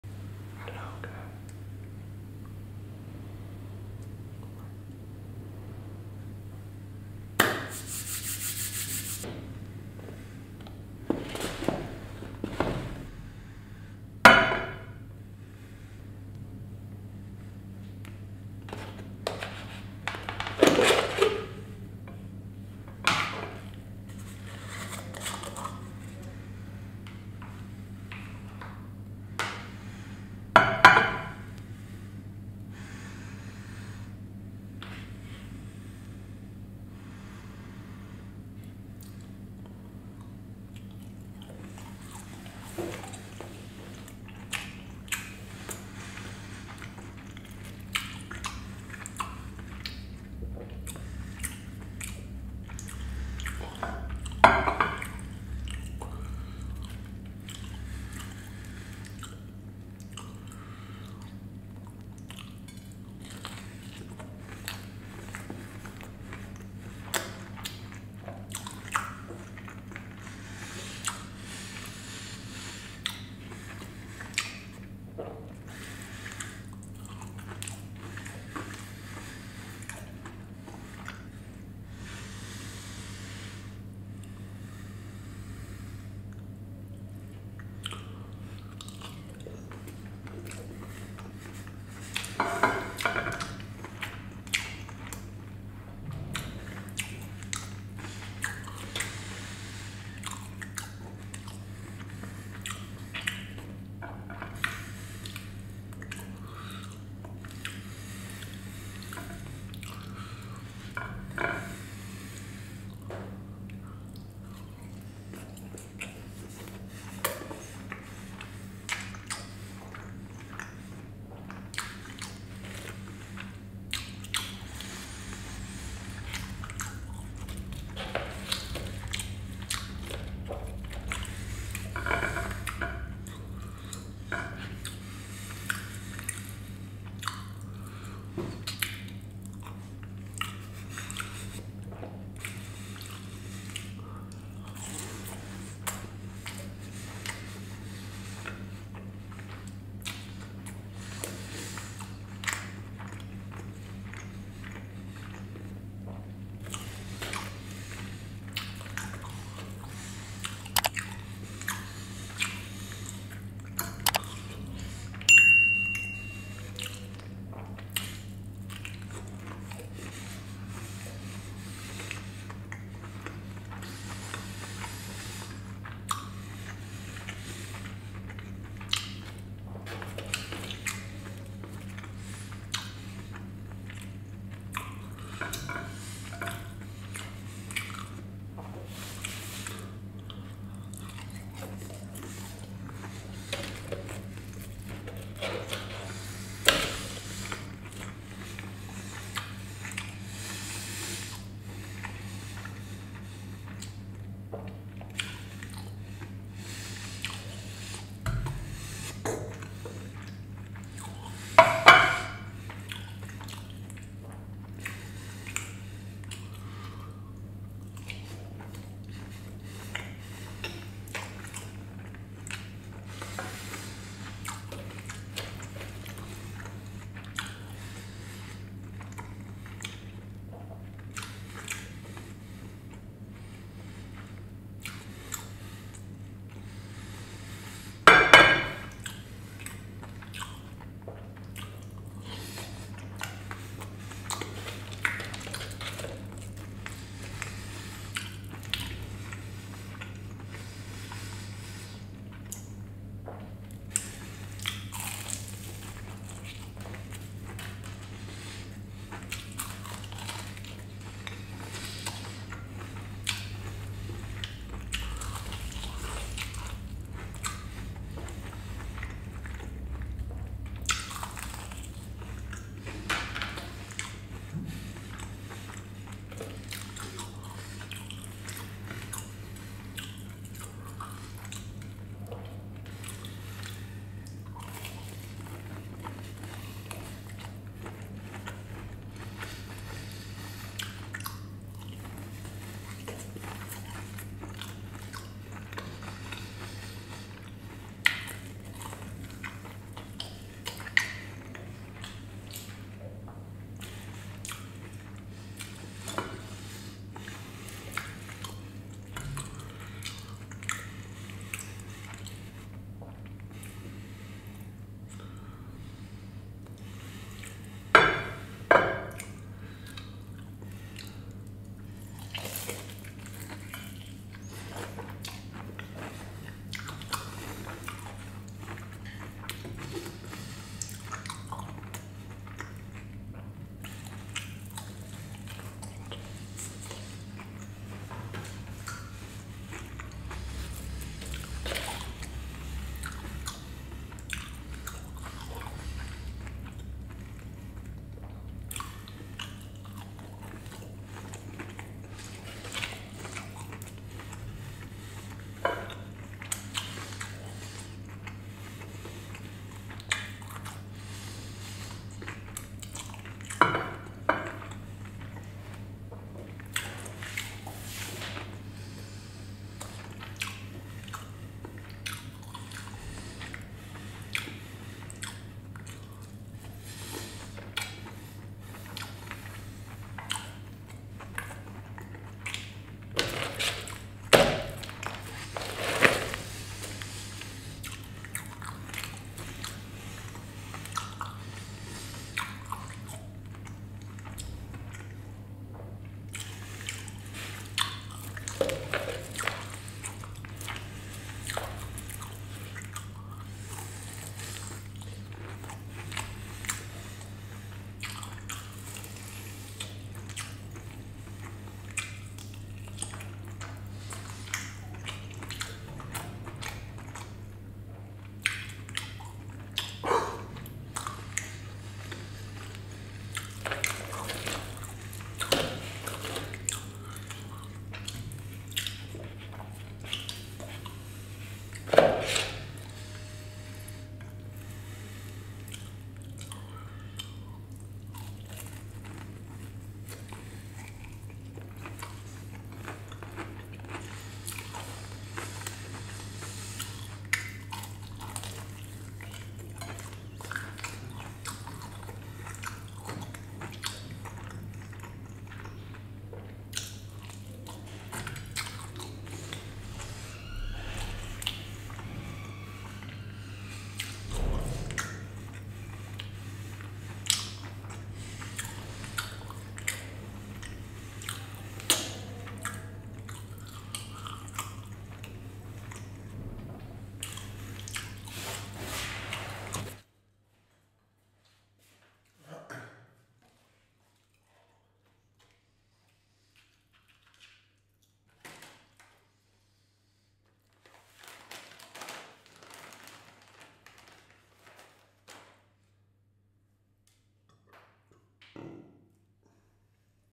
ASMR KFC WITH NUTELLA | WEIRD FOOD COMBOS
In this video, for the first time in my life, i'm going to eat Kfc With Nutella!
I like to mix flavours and i will do it more often!

피자 먹기